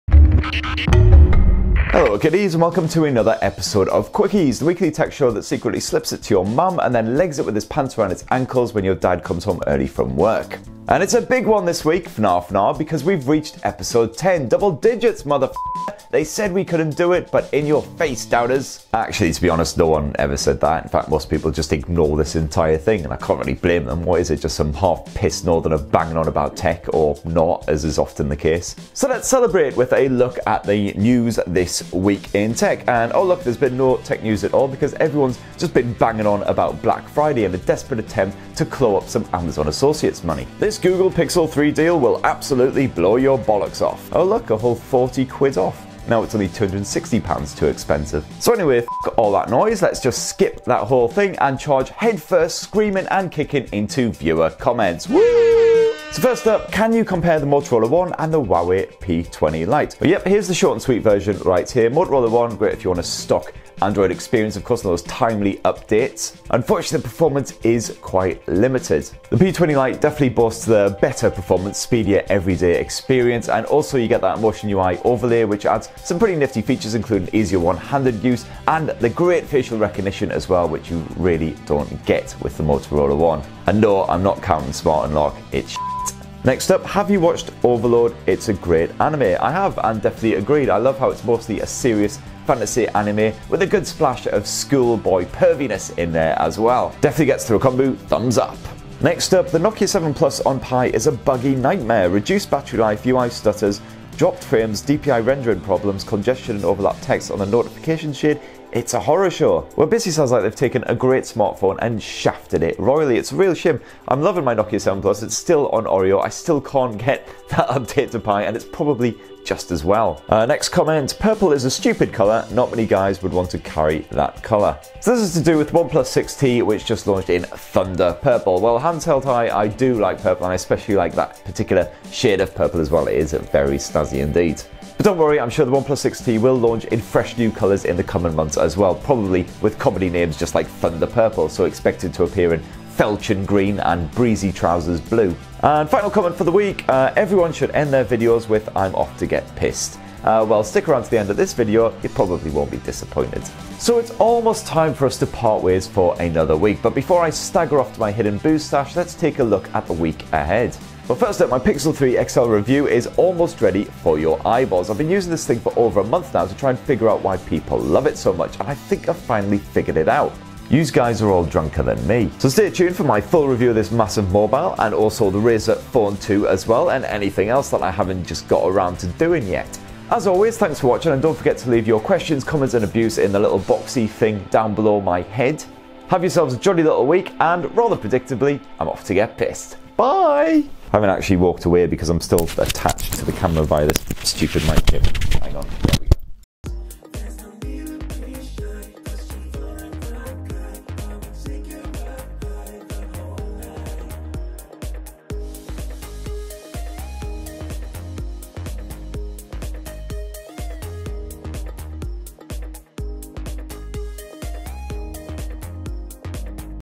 Quickies Ep10 | Black Friday, Moto One vs P20 Lite, Nokia 7 Plus on Pie
On this week's Quickies we discuss Black Friday 'deals' on the likes of Google's Pixel, which are about as thrilling as a dead fish's wet bits. In viewer comments we compare the Motorola One and Huawei's P20 Lite, comment on the Thunder Purple OnePlus 6T, there's more on the Nokia 7 Plus' Android Pie woes, and more besides.